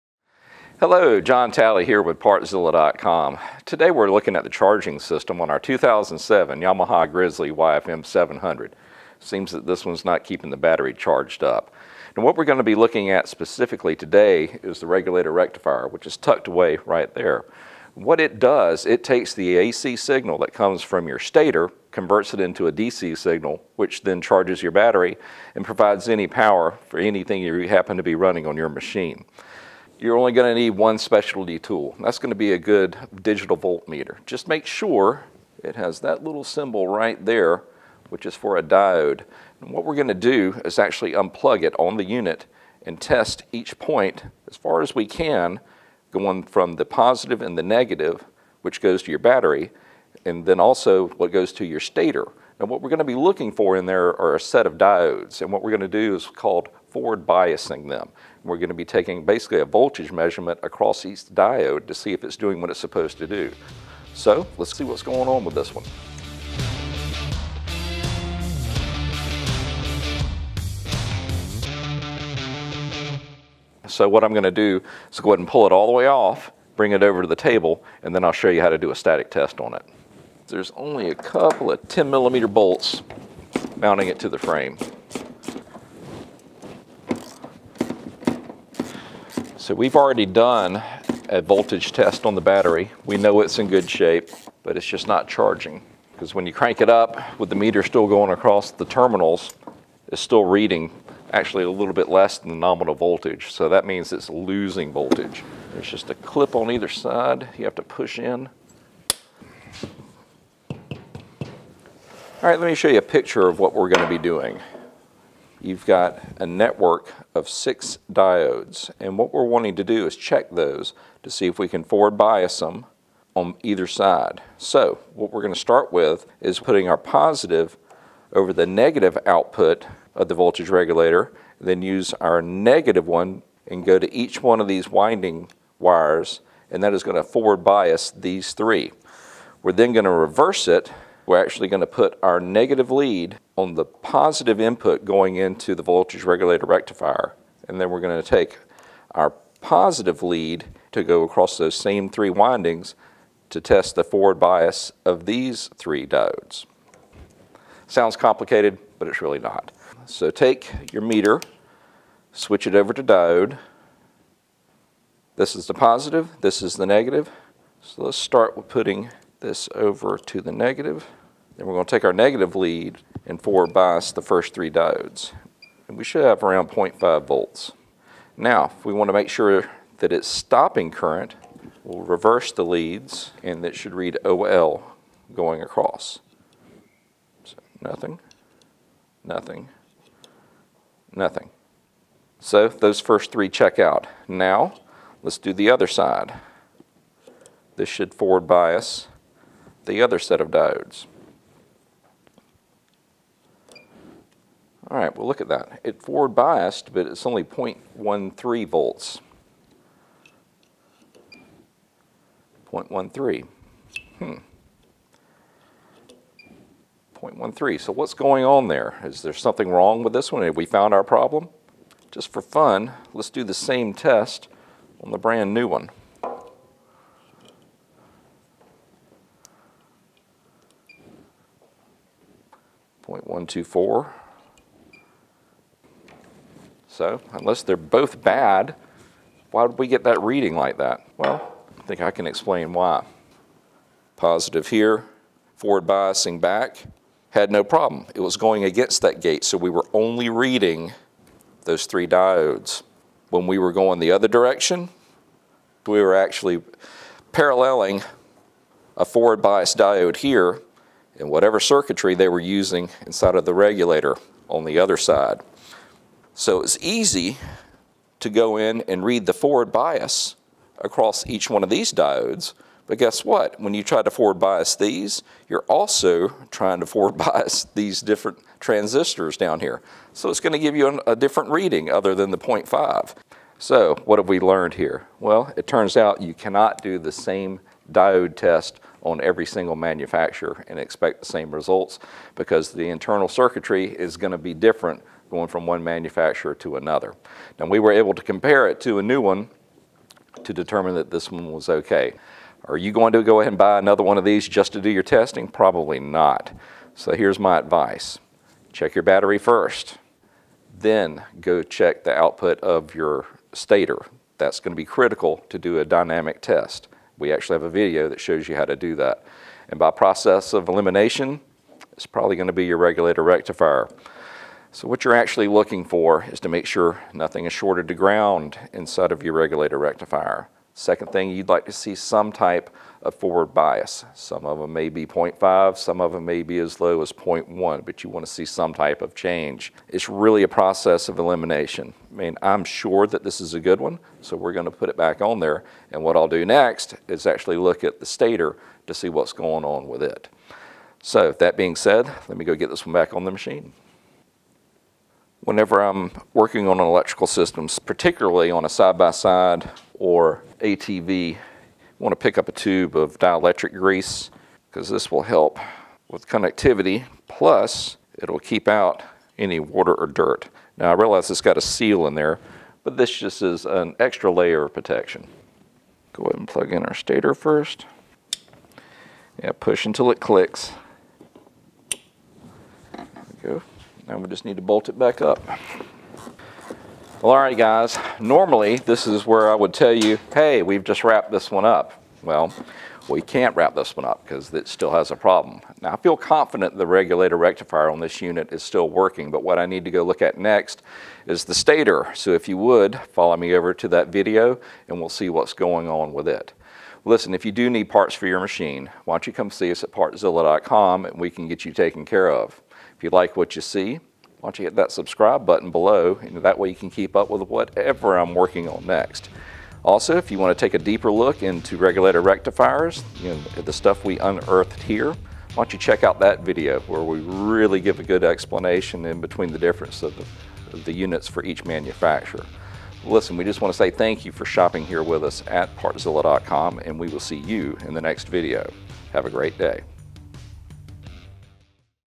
Yamaha Grizzly Not Charging | ATV Charging Problems | Partzilla.com
We’re investigating charging problems on our Yamaha Grizzly, and today we’re going to test the regulator rectifier on our Yamaha ATV. John will show you how to disconnect the regulator rectifier from the unit, and then he’ll guide you through how to test the regulator rectifier on your ATV. Then, we’ll get the rectifier back on the unit and tell you about what’s next as we run down the charging problems on our ATV.

Want a closer look? Check out a diagram of the electrical system on your Yamaha Grizzly:

Read more about how to diagnose charging problems in your Yamaha Grizzly step-by-step

VIDEO HIGHLIGHTS:
Removing the regulator rectifier - 1:15
Testing the regulator rectifier - 1:50
Reconnecting the regulator rectifier - 5:55

In the video we’re working with a 2007 Yamaha YFM700 Grizzly, but you may have similar steps for your Yamaha ATV.